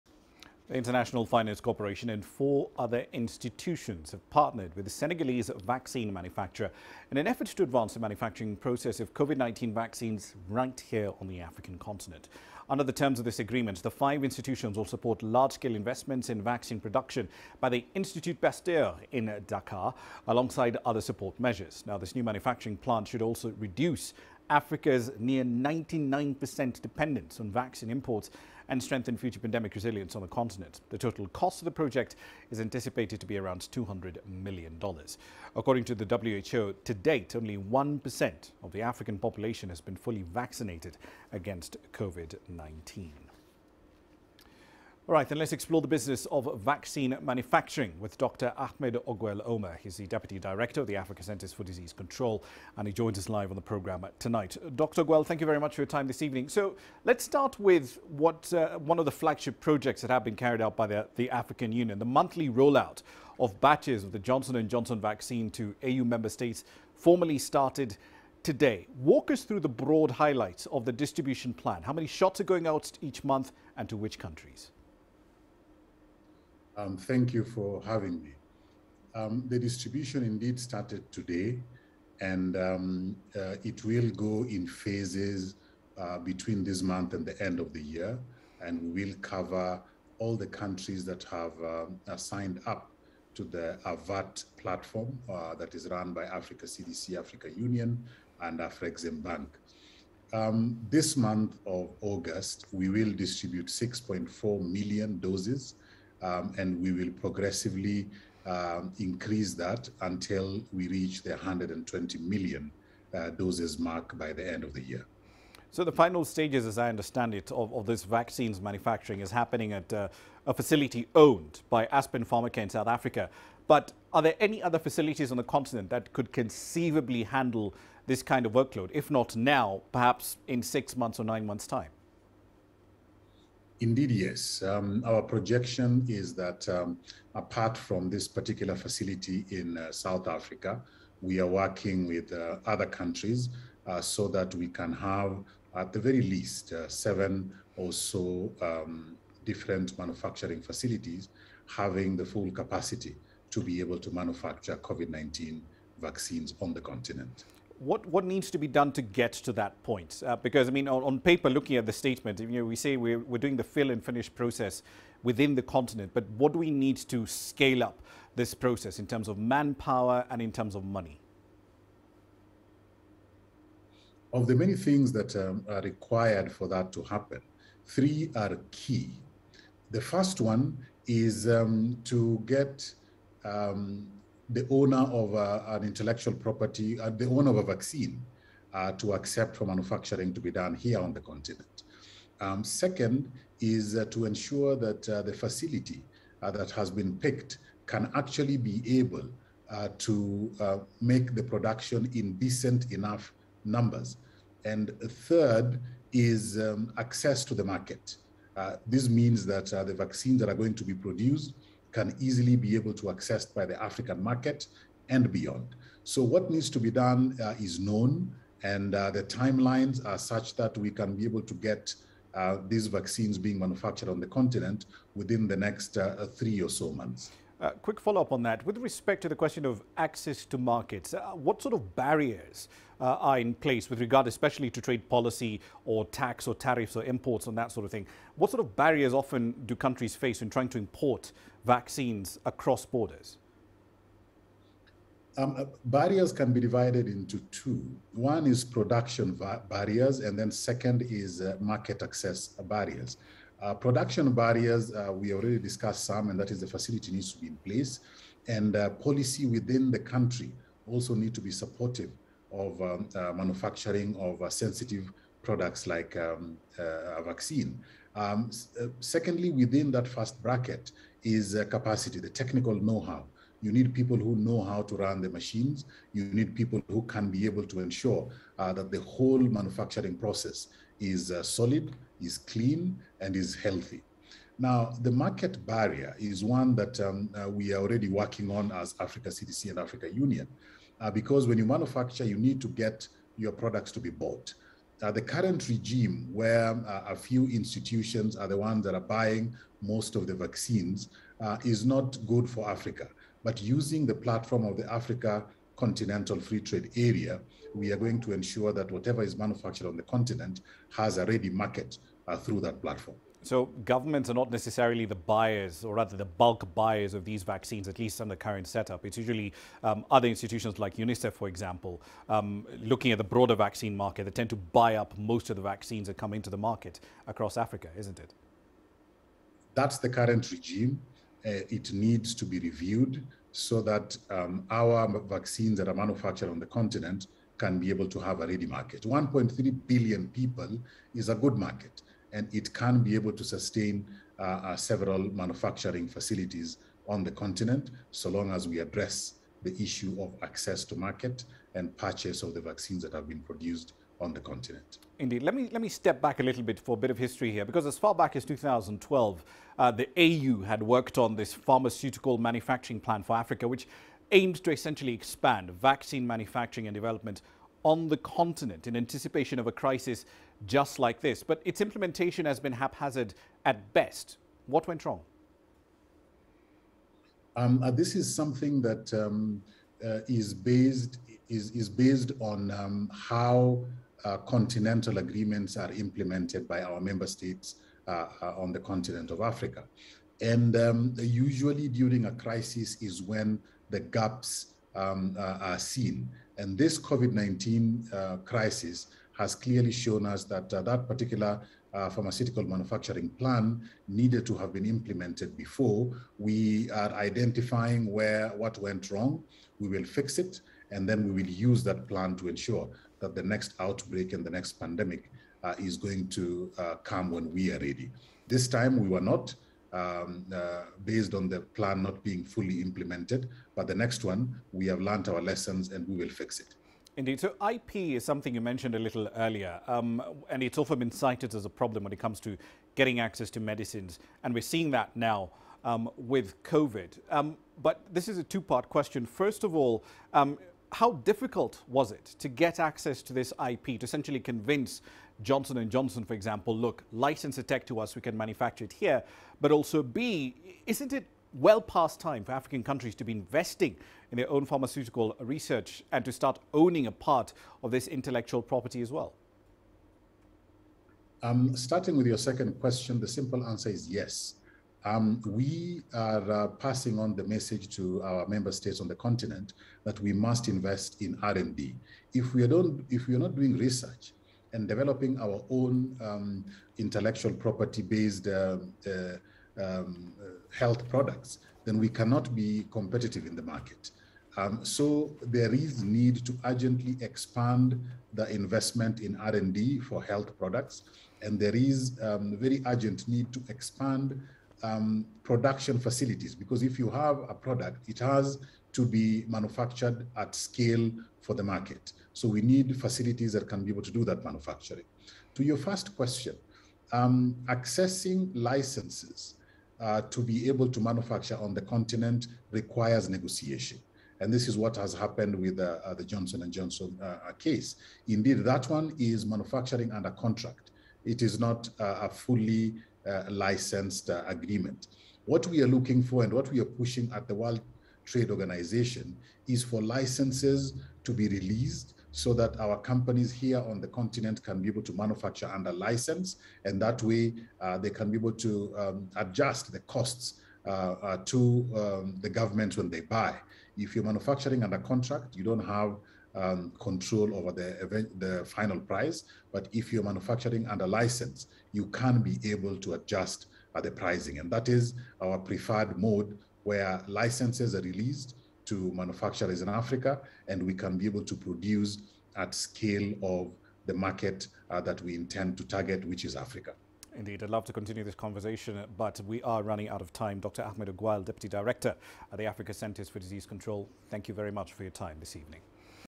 Vaccine rollout gets international financial support
The International Finance Corporation and four other institutions have partnered with a Senegalese vaccine manufacturer in an effort to advance the manufacturing process of COVID-19 vaccines in Africa. Under the collaboration agreement, the 5 institutions will support large-scale investment in vaccine production by the Institut Pasteur in Dakar, alongside other support measures. The new manufacturing plant should reduce Africa's 99% dependence on vaccine imports and strengthen future pandemic resilience on the continent. The total cost of the project is anticipated to be $200 million. According to the World Health Organisation, to date, only 1% of the African population has been fully vaccinated against COVID-19.